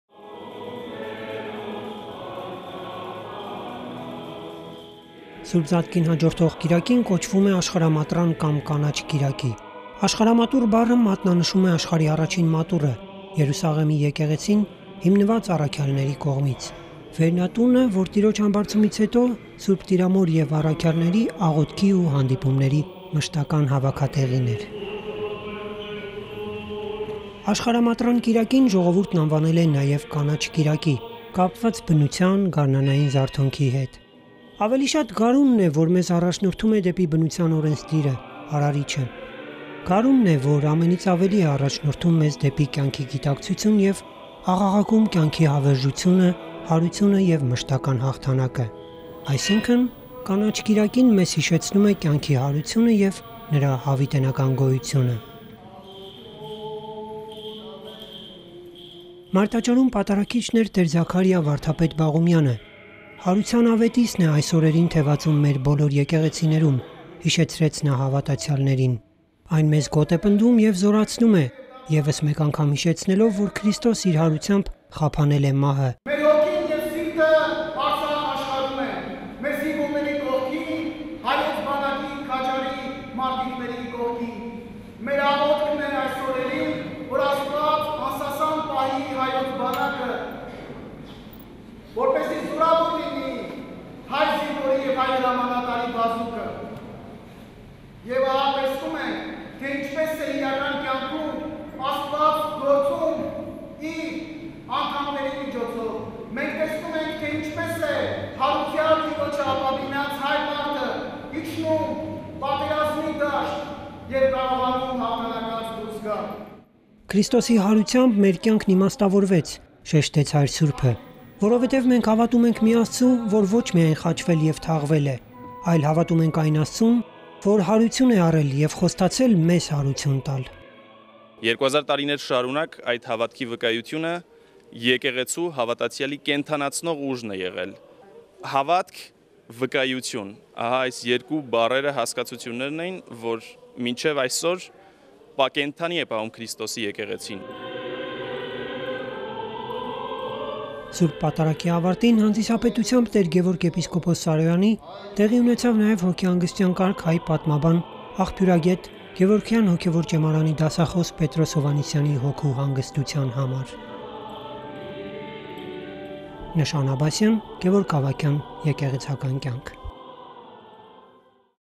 Sunday of the World Church (Green Sunday)
The second Sunday following the Glorious Resurrection of Our Lord Jesus Christ is the Sunday of the World Church. It commemorates the first Church of Jerusalem, established by Christ. Following Divine Liturgy in the Mother Cathedral of Holy Etchmiadzin, a Repose of Soul Service was offered for professor and academic Mr. Petros Hovhannisyan.

Աշխարհամատրան կիրակի (Կանաչ կիրակի)

Սբ. Զատկին հաջորդող երկրորդ կիրակին կոչվում է Աշխարհամատրան կիրակի՝ ի հիշատակ Երուսաղեմում  առաջին եկեղեցու հիմնադրման: Ս. Էջմիածնի Մայր Տաճարում, հավարտ Ս. Պատարագի, կատարվեց հոգեհանգստյան արարողություն պատմաբան Պետրոս Հովհաննիսյանի հոգու հանգստության համար: